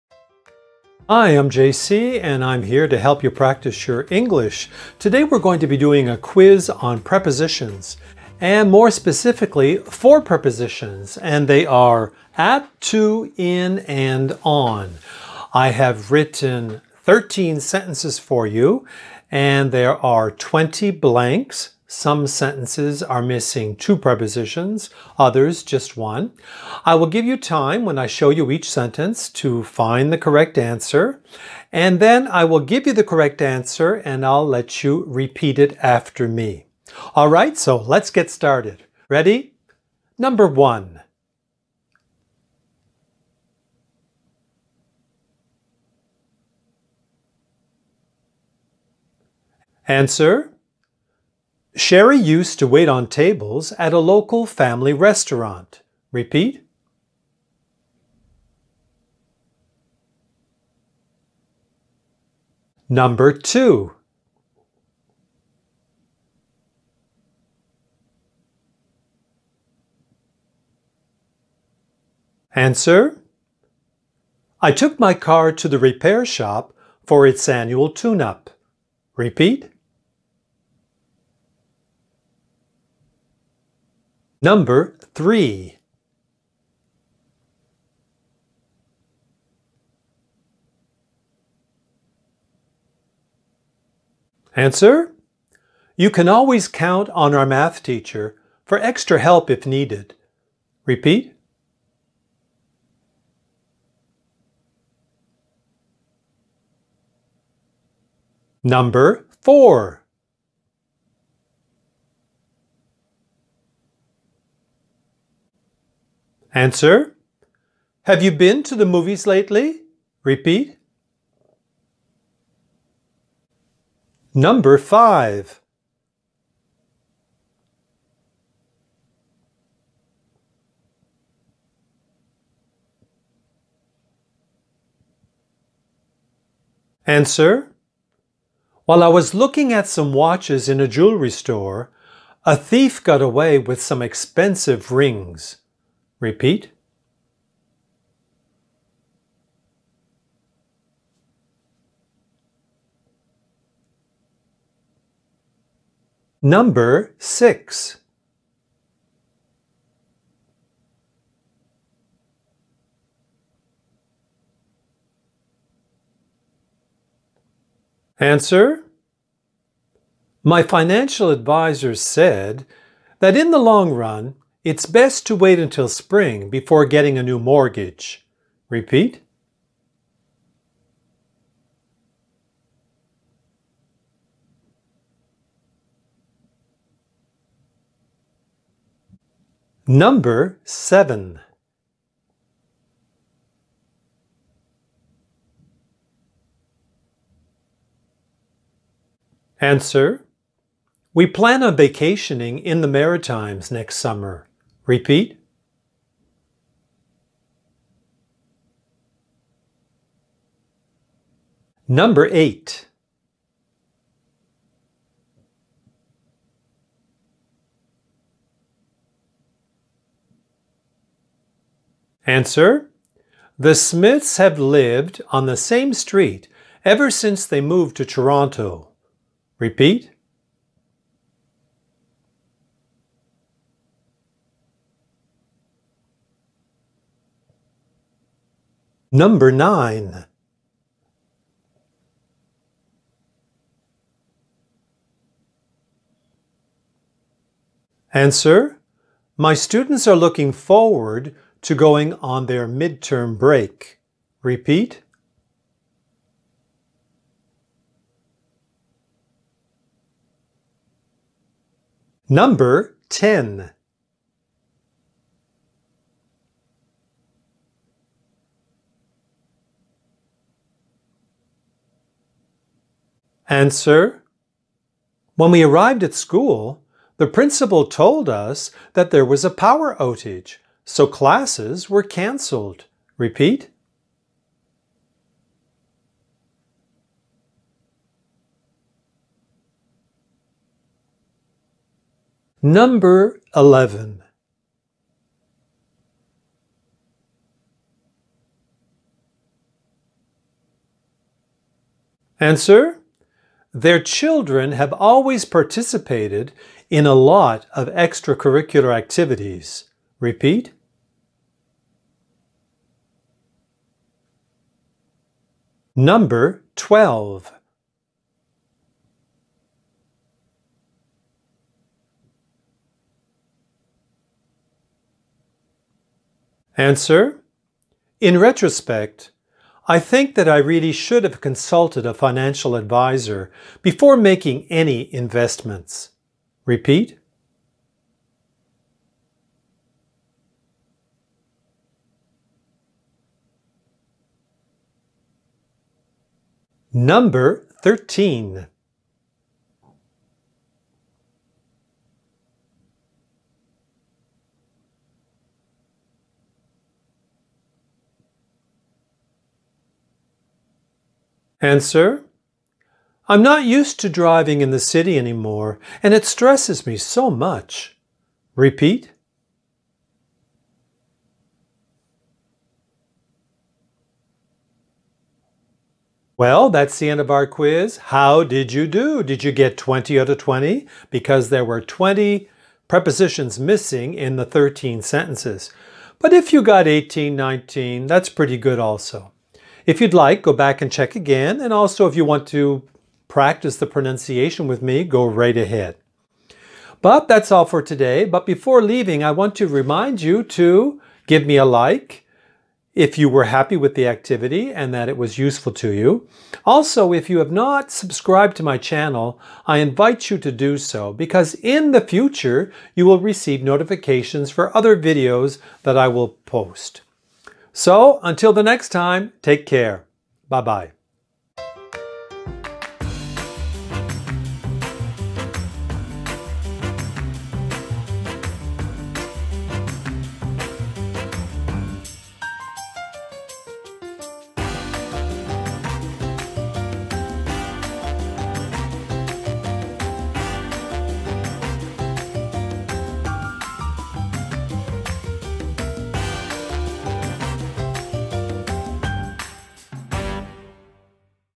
English Prepositions: IN, ON, TO, & AT (B Level)
This video will test students ability to use the prepositions IN, ON, TO, and AT correctly in sentences shown by your English teacher, JC.

After students have decided which preposition is needed for each blank, JC will provide the correct answer orally and students will have the time to repeat after him for pronunciation practice.